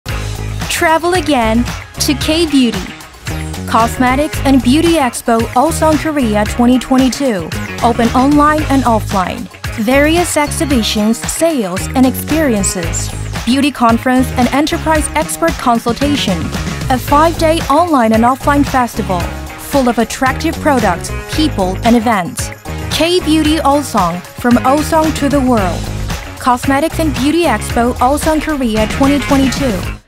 Cosmetics&Beauty Expo, Osong Korea 2022ㅣkbeutyexpo l koreabeauty l lbeautyexpo l osongbeautyexpo
-Date: October 18 ~ 22, 2022 (5Days)
-Organized/Conducted by: Chungcheongbuk-do and Cheongju-si/Osong Bio Promotion Foundation
-Contents: Online Industrial Exhibitions, Online Busin ess Meeting, K-beauty Exhibitions, e-Conferences, and more